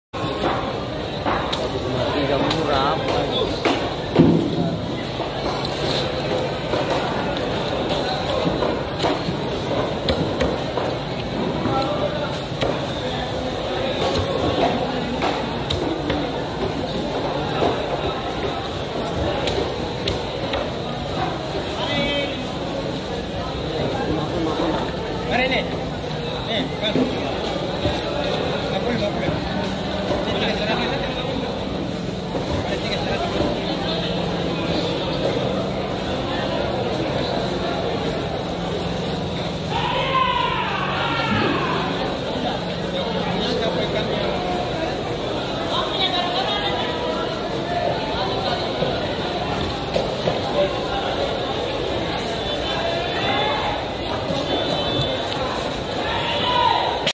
Amazing cutting Skills Big Tuna
Assalamualaikum warohmatullahi wabarokatuh, selamat berjumpa di Channel Abang Fadil Official

Abang Fadil Official Akan menyajikan video menarik tentang skill para pemotong ikan dan berbagai aksi para penjual ikan dalam menawarkan jenis ikan yang di jual di pasar ikan jembatan puri kota Sorong Papua Barat Daya Indonesia
Hormat Kami Abang Fadil Official Channel